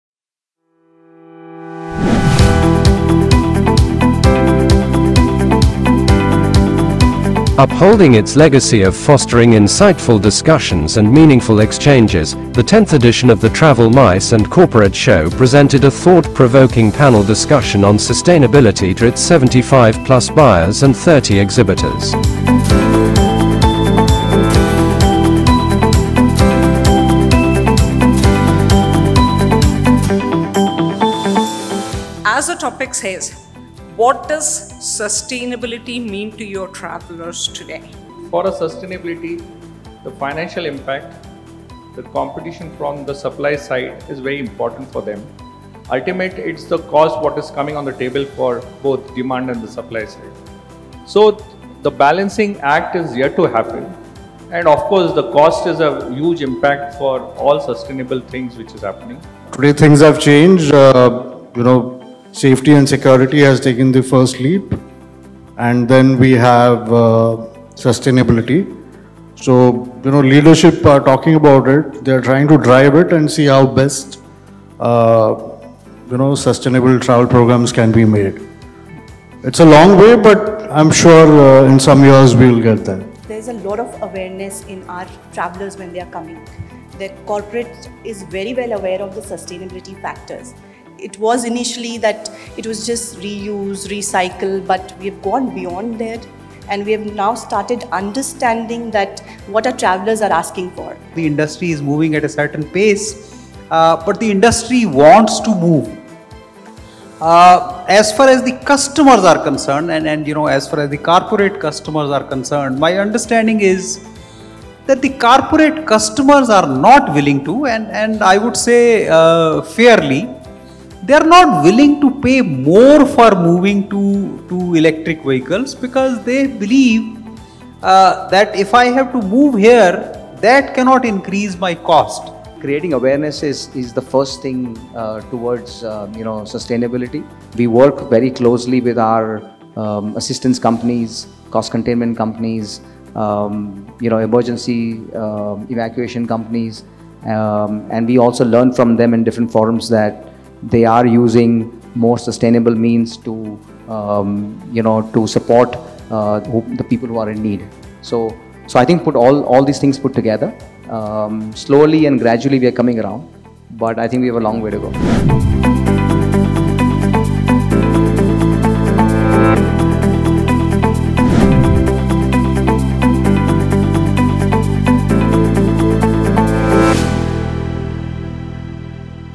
10th Travel MICE & Corporate Show - Part 1
10th Travel MICE & Corporate Show - Part 1#travel

Upholding its legacy of fostering insightful discussions and meaningful exchanges, the 10th edition of the Travel MICE & Corporate Show presented a thought-provoking panel discussion on sustainability to its 75 + buyers and 30 exhibitors.

 we Interviewed

- JYOTHI VARMA, Advisor , Travel MICE and Corporate Show
- SANJAY PAI, Vice President & Head, Corporate Travel, Aviation and Facility Management, Larsen & Toubro
- GAURAV NAGWEKAR, Head-Corporate Travel, Reliance Industries
- NEHA BOSE, Hotel Manager, The Westin Hyderabad Hitec City
- RAJIV VIJ, Chairman and Managing Director, Carz on Rent
- DEV KARVAT, Founder & CEO, Asego